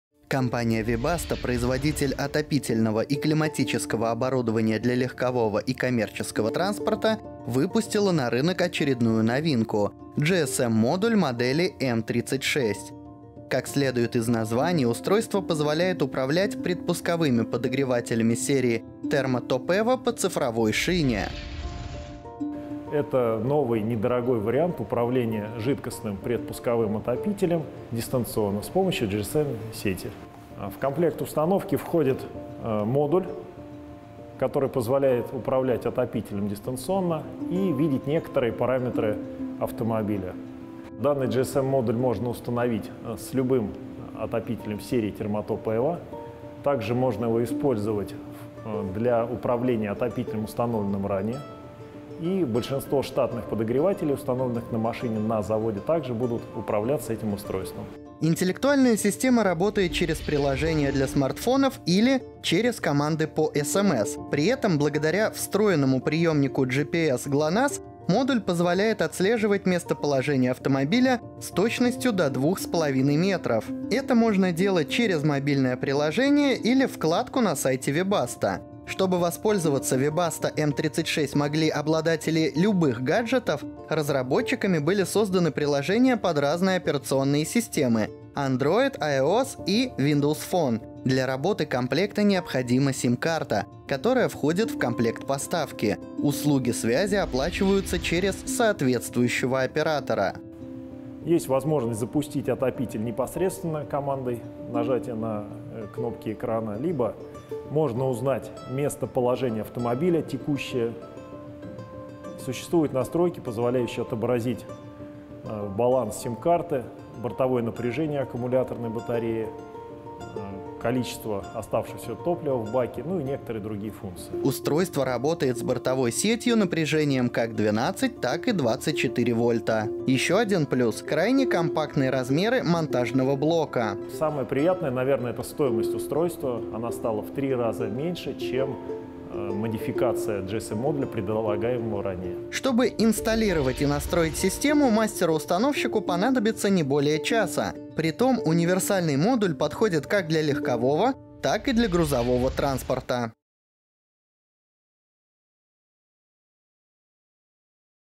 Webasto М36 позволит управлять отопителем дистанционно | Новости с колёс №1251
Компания Webasto, производитель отопительного и климатического оборудования для легкового и коммерческого транспорта, выпустила на рынок GSM-модуль модели M36. Устройство позволяет управлять предпусковыми подогревателями серии Thermo Top Evo по цифровой шине.